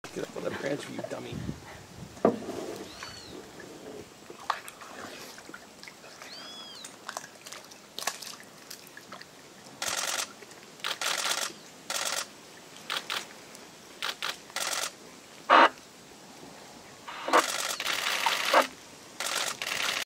GIANT RIVER OTTER - CLOSE UP AT THE PANTANAL! ARIRANHA DE PERTINHO NO PANTANAL!
It is incredible how close we can get to them!